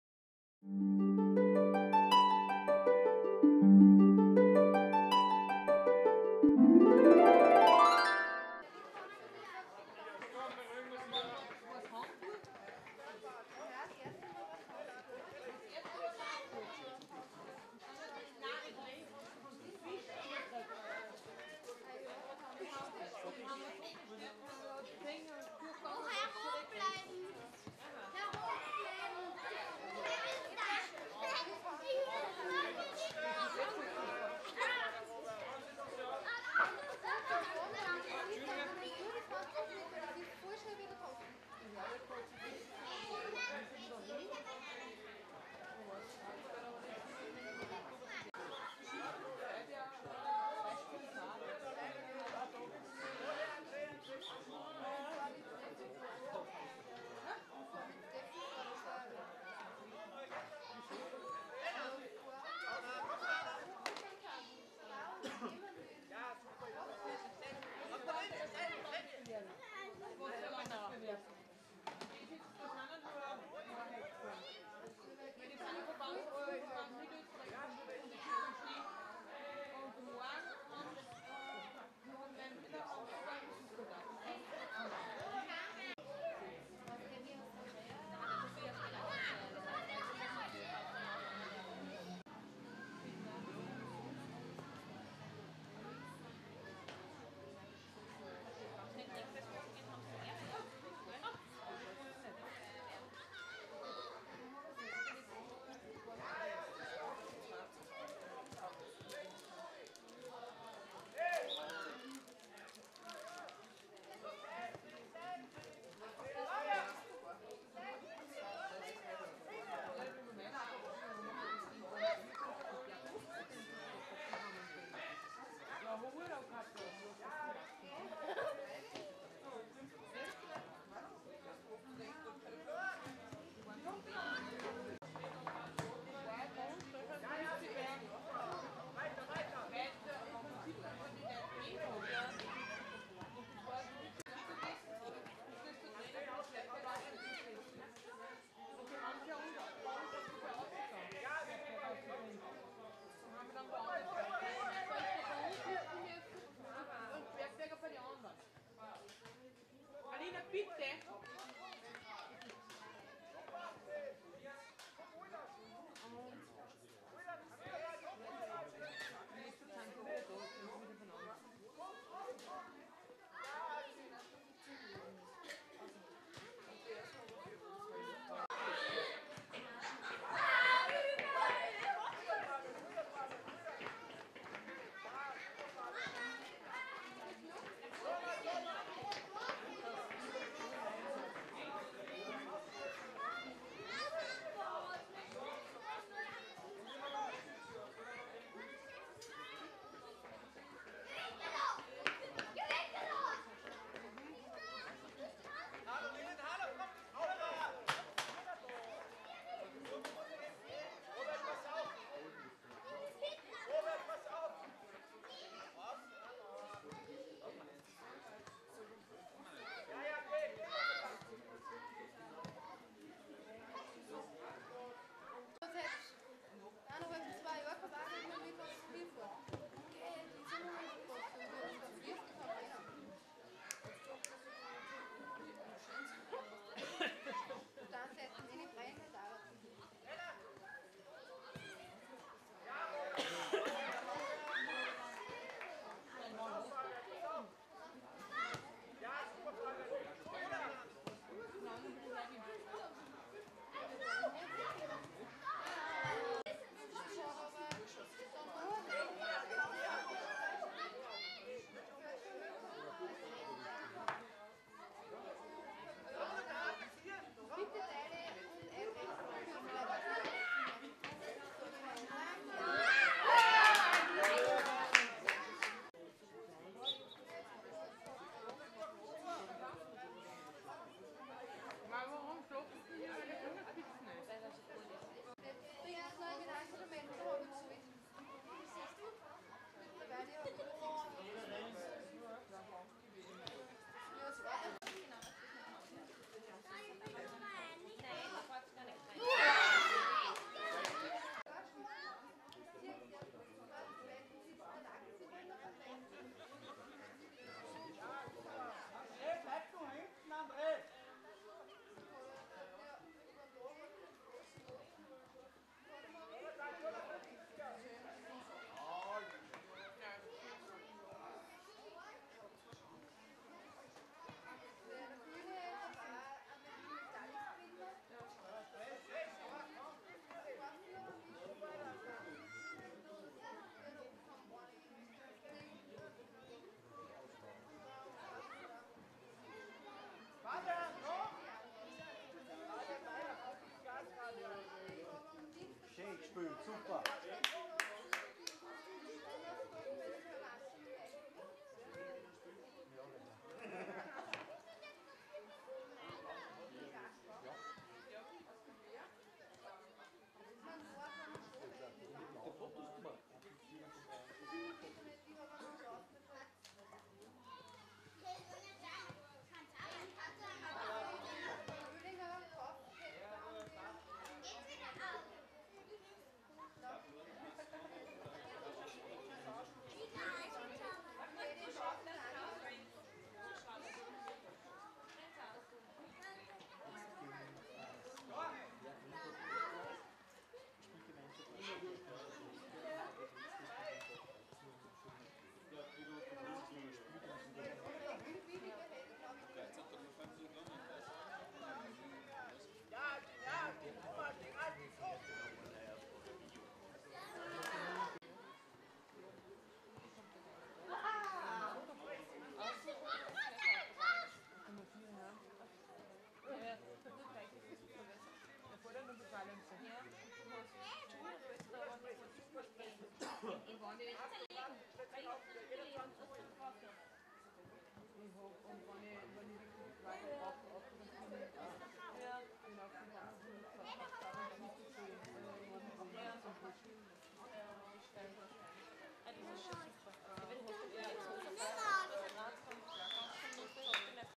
Bad Kreuzen || SPG A. Perg-Windhaag-Rechberg A
U-9 Nachwuchsturnier in Bad Kreuzen
Bad Kreuzen gegen SPG A. Perg-Windhaag-Rechberg A (Ergebnis: 2:0)